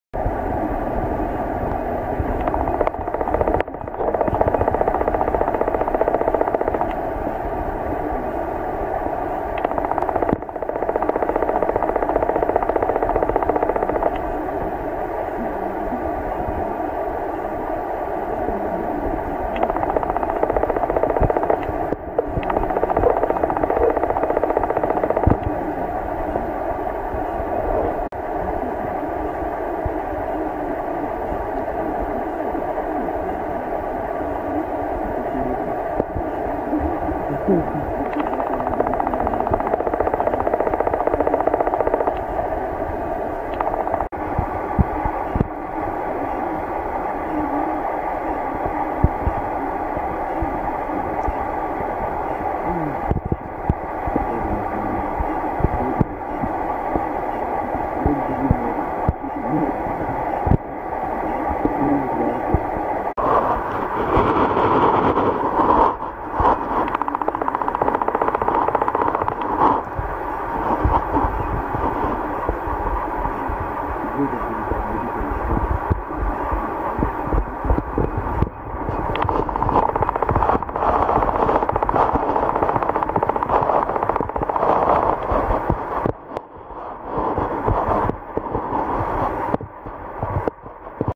Мелкие рыбёшки и мальки реки Быстрицы
Мелкие рыбёшки и мальки плавают на песчаном мелководье реки быстрицы, роются в песке